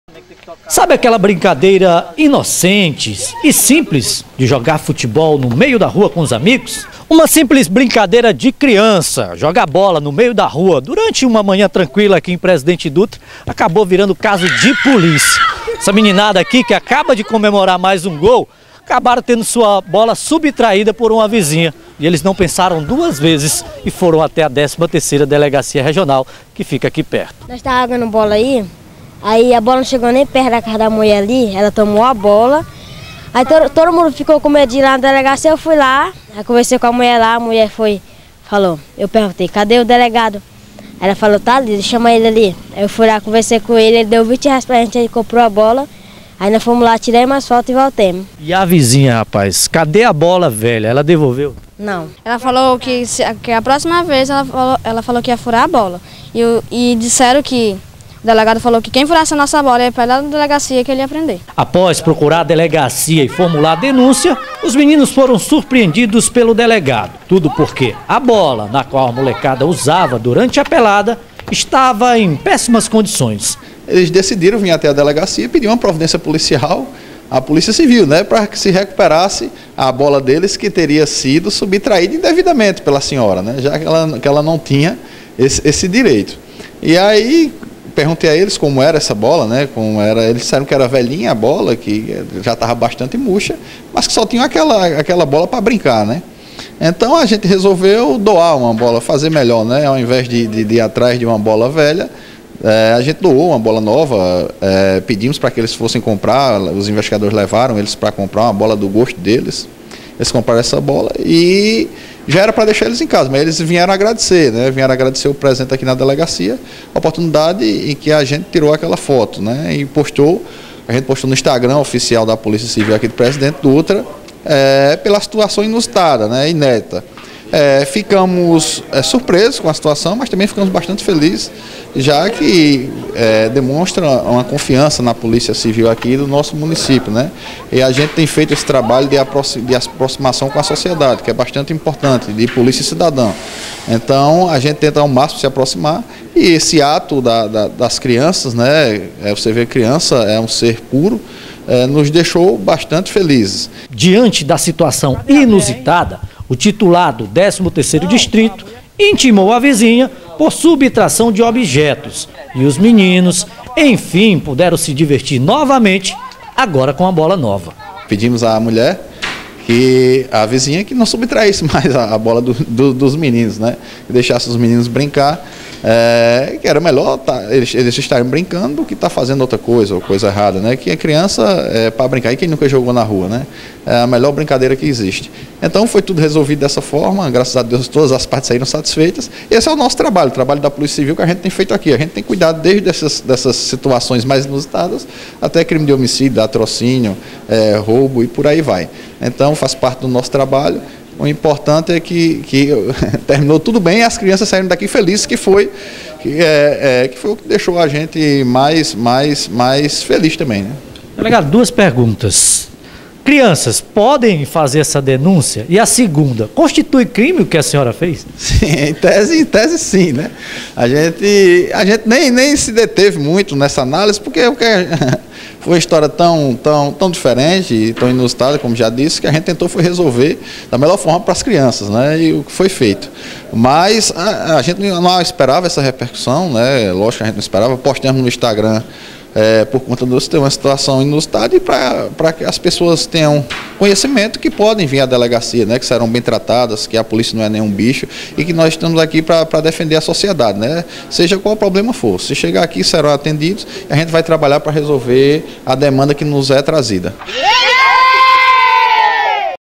CRIANÇAS PROCURAM A DELEGACIA DE POLICIA CIVIL APÓS VIZINHA TOMAR BOLA EM PRESIDENTE DUTRA - MA
CRIANÇAS PROCURAM A DELEGACIA REGIONAL DE POLICIA CIVIL APÓS VIZINHA TOMAR A SUA BOLA POR CAUSA DE TER BATIDO NA SUA PORTA EM PRESIDENTE DUTRA - MA

Ontem (dia 9) essa turma procurou a Polícia Civil e informou que estava jogando bola na rua, oportunidade em que a vizinha pegou a bola por ter batido em sua porta.

Assim, pediram providência policial em razão da Subtração rsrsrsrs. Relataram, ainda, que a bola estava bastante velha e murcha, mas era a única que tinham. Comovida com a história, a Polícia Civil doou uma bola nova e intimou a vizinha para que não subtraia mais a bola. 😂😂😂

Copyright@2019

Compartilhe !!!

REPÓRTER: ADONIAS SOARES E DÉBORA CARVALHO
EDIÇÃO DE IMAGENS: AYRTON SILVA E ISRAEL DELAMARK

2019